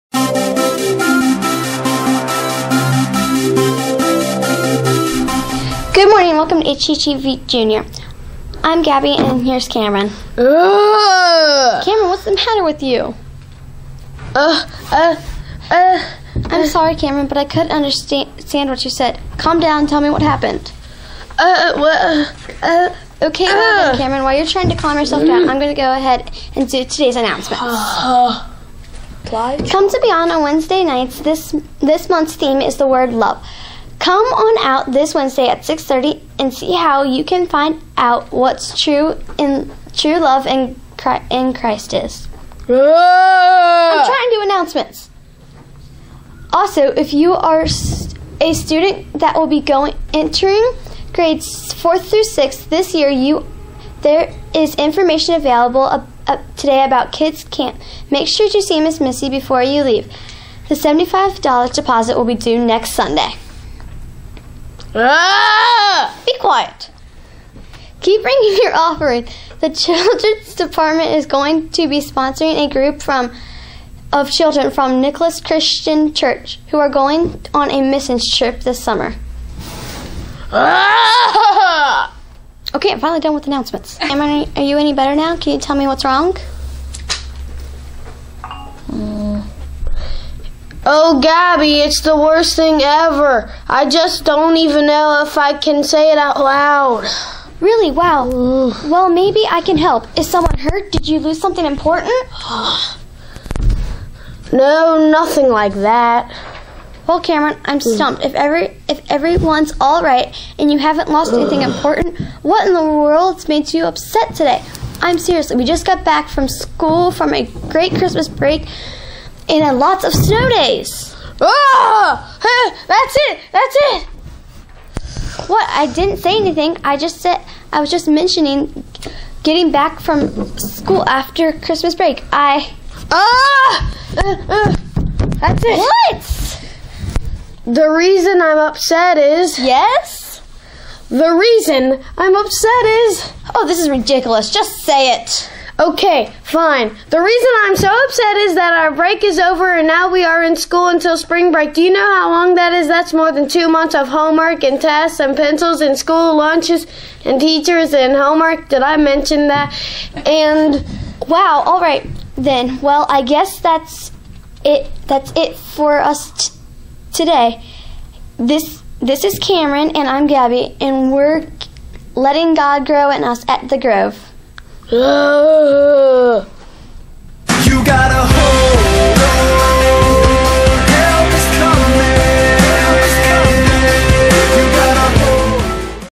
HGTV Jr. - Jan. 30, 2011
The weekly announcement video for our children's ministry at Hickory Grove Baptist Church in Independence, KY!